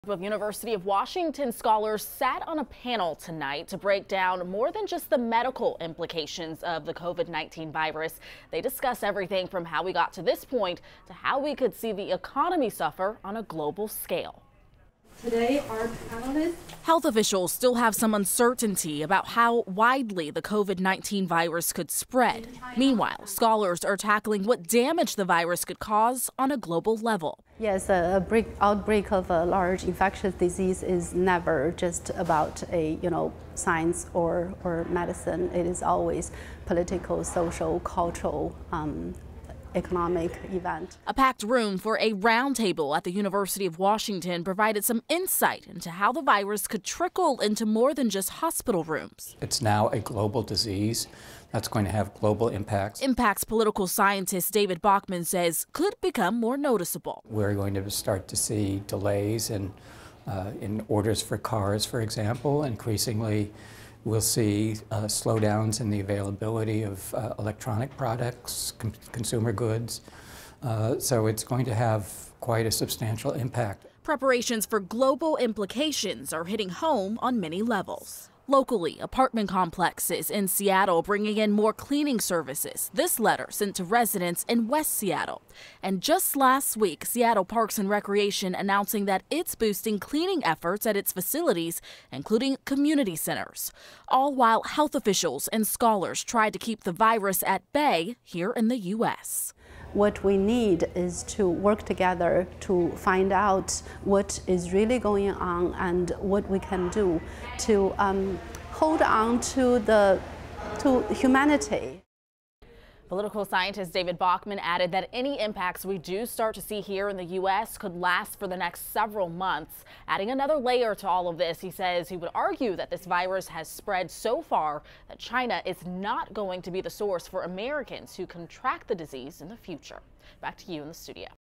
UW scholars discuss the kind of damage COVID-19 could have on a global scale
The impacts go beyond the hospital-- a virus like this can impact the economy and consumer goods.